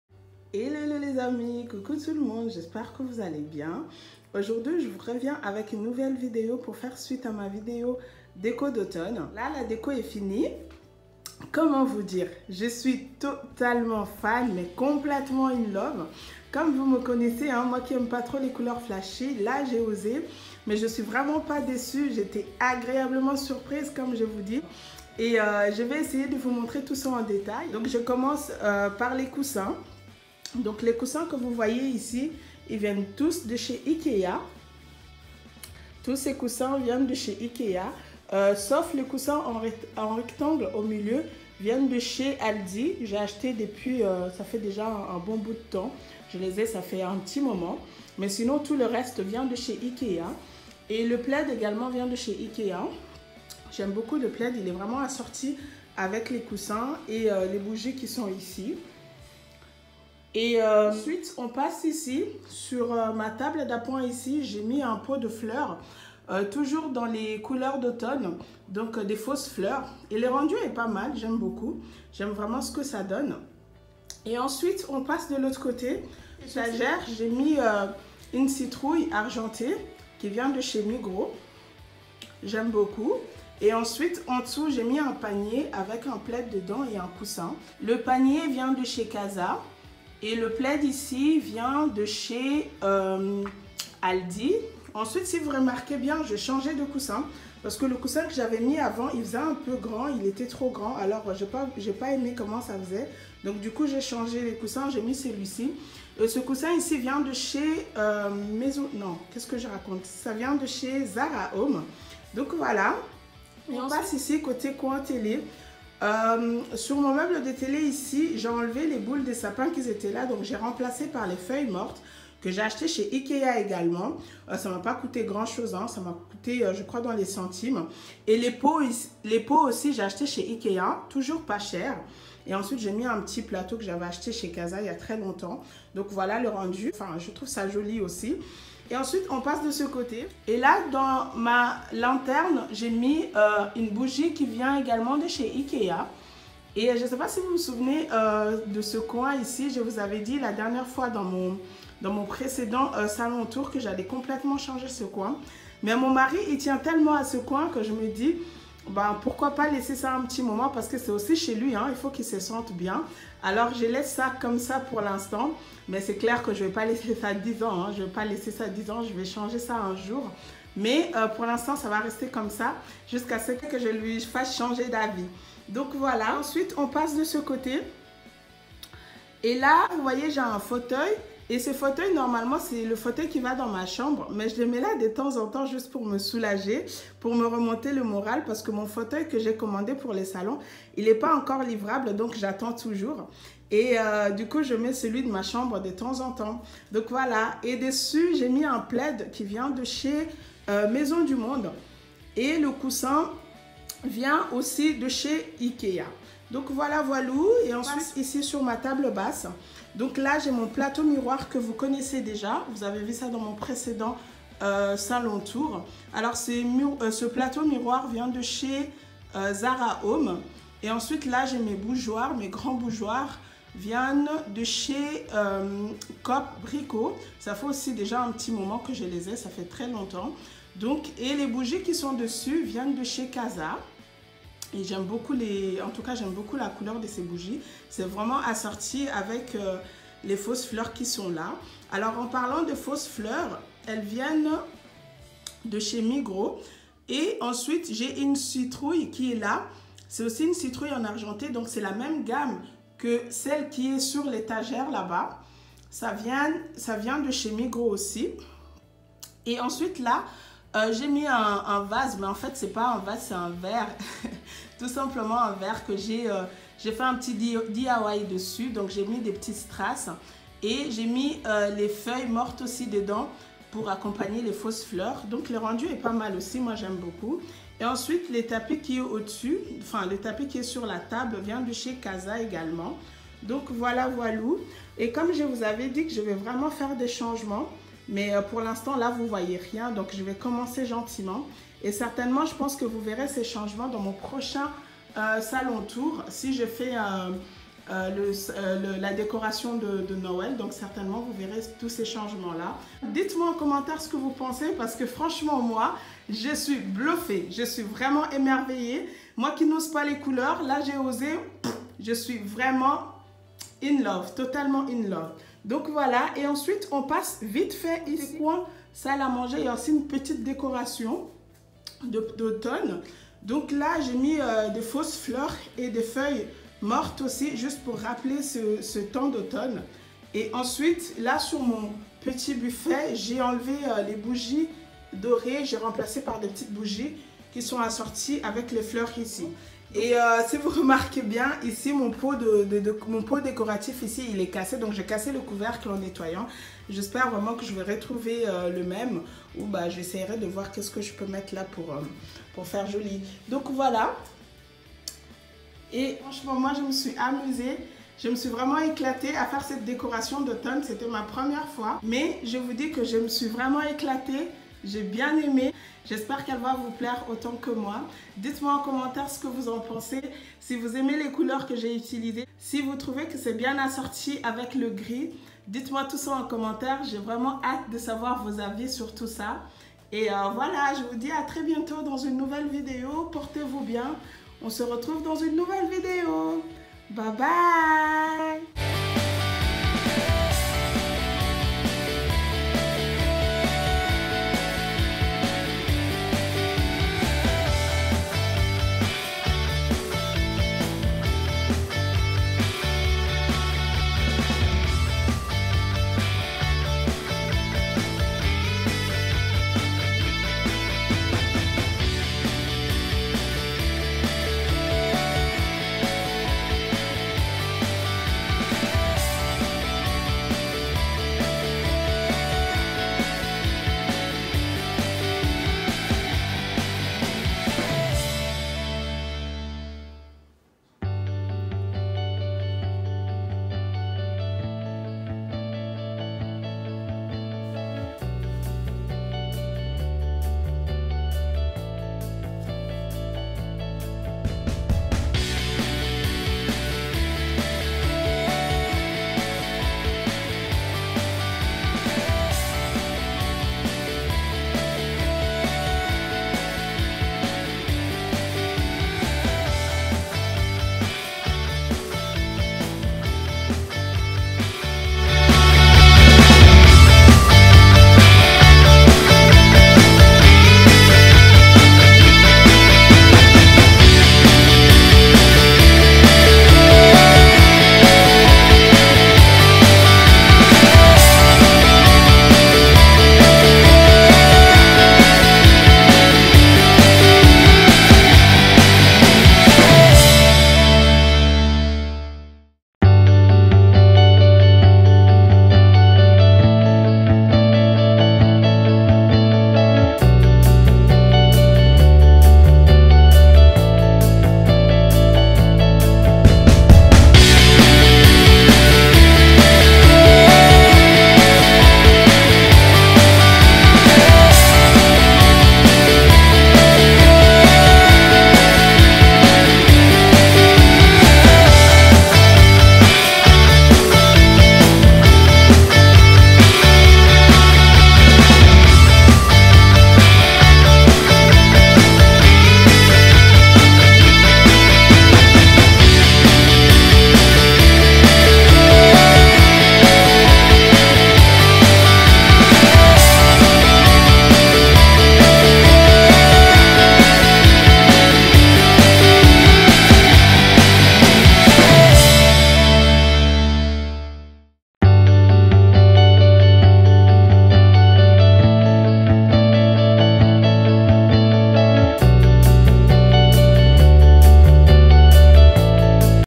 SALON TOUR, DÉCORATION D’AUTOMNE avec les détails | LIVING ROOM TOUR: my fall decor tour 2018
Coucou tout le monde, je reviens avec une nouvelle vidéo SALON TOUR DÉCO D’AUTOMNE, j’espère qu’elle vous plaira. Si c’est le cas n’hésitez pas à like, à commenter et SURTOUT À VOUS DONNER, c’est complètement GRATUIT